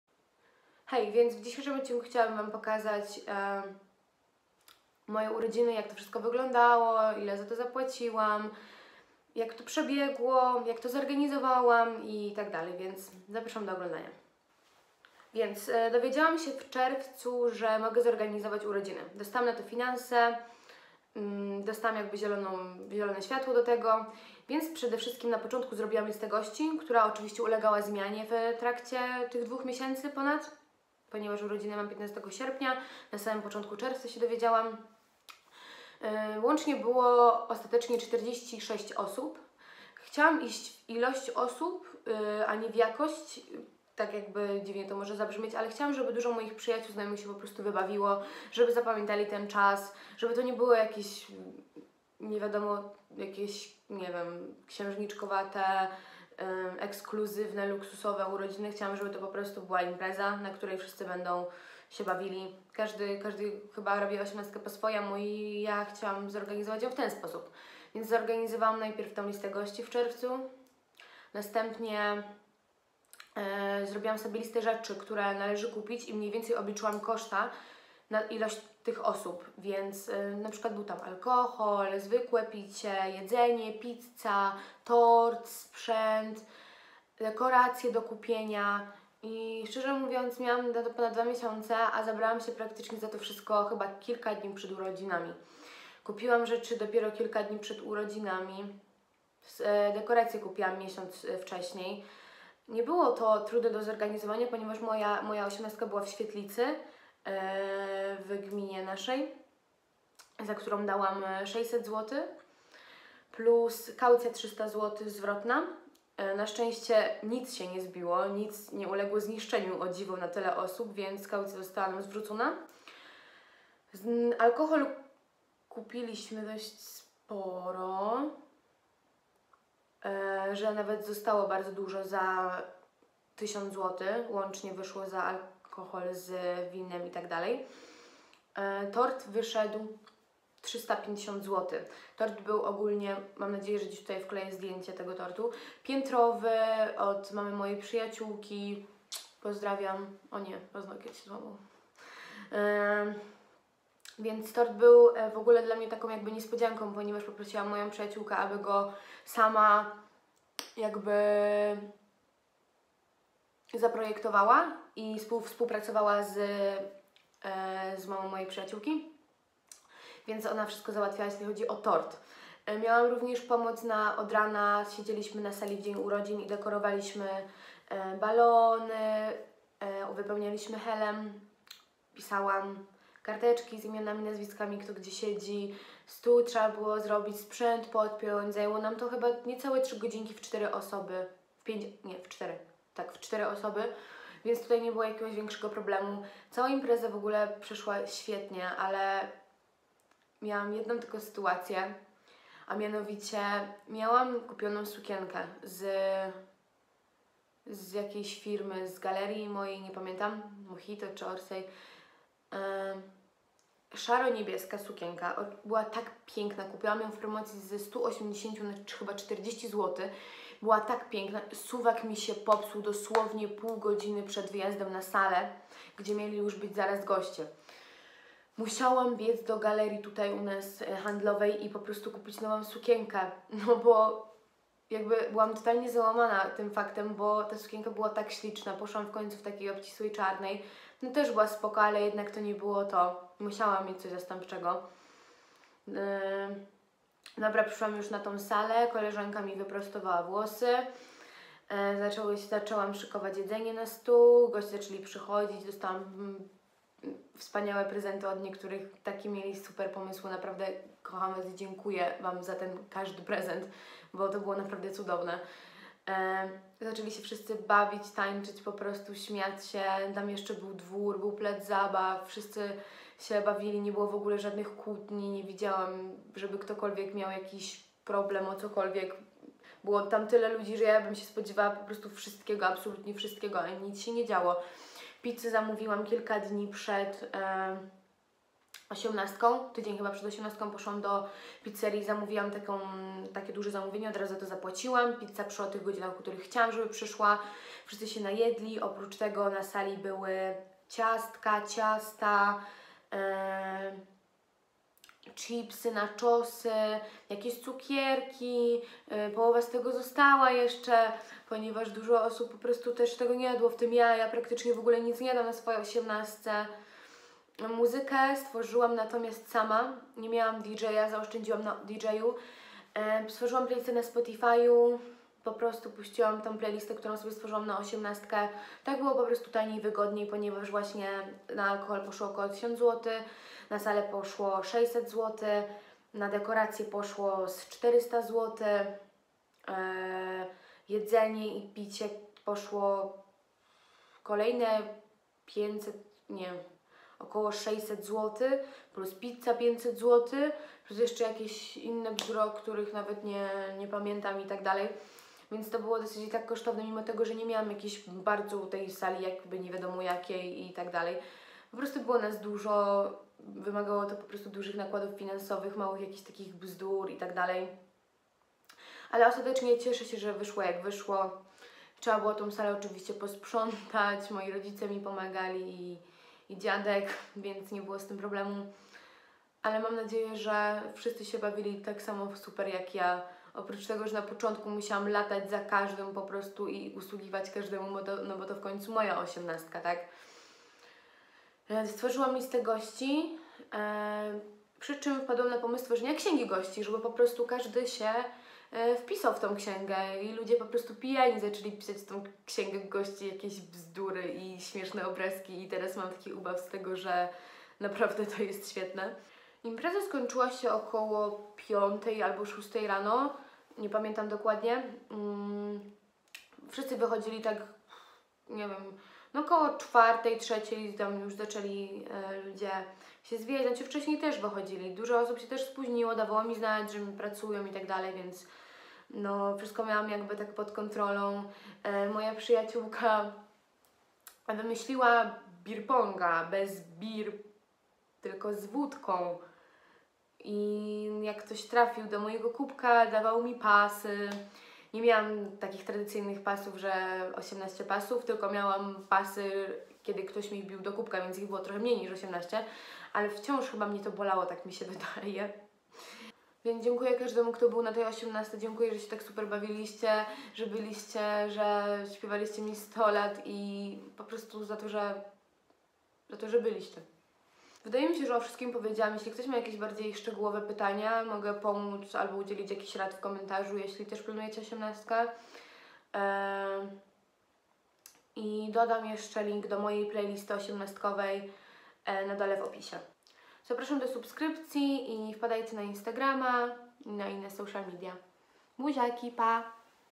Moja 18-stka!
gdzie, co, jak, gdzie, kiedy? Krótki film o moich 18-stych urodzinach.